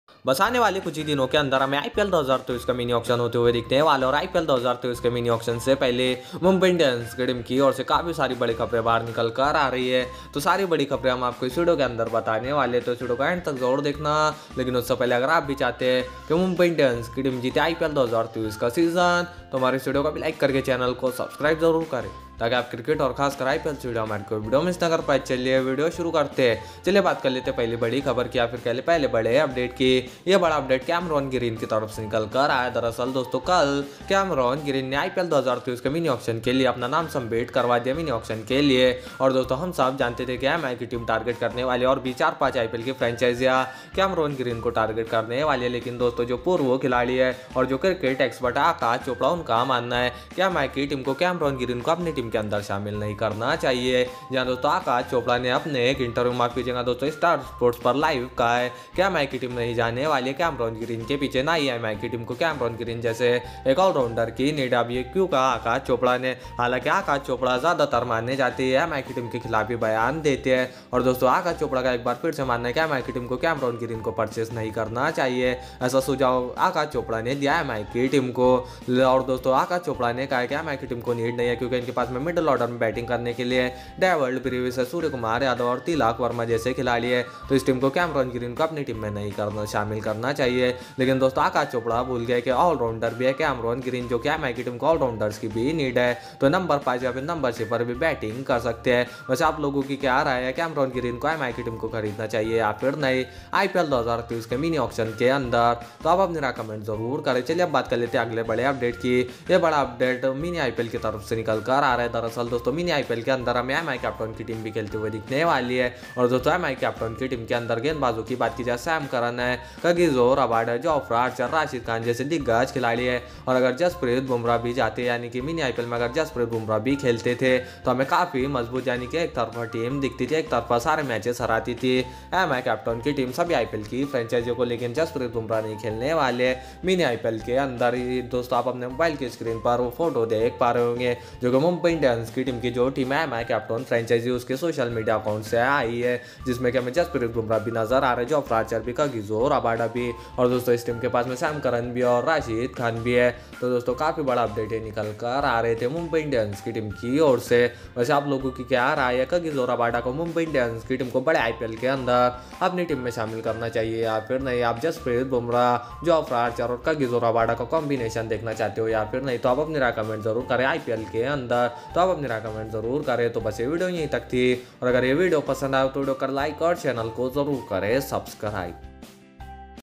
IPL 2023 - MI Will Not Buy Cameron Green | MI Team News | MI Target Players 2023 Mini Auction
THIS VIDEO RELATED FOR MUMBAI INDIANS IPL 2023 AUCTION STRATEGY & TARGET PLAYERS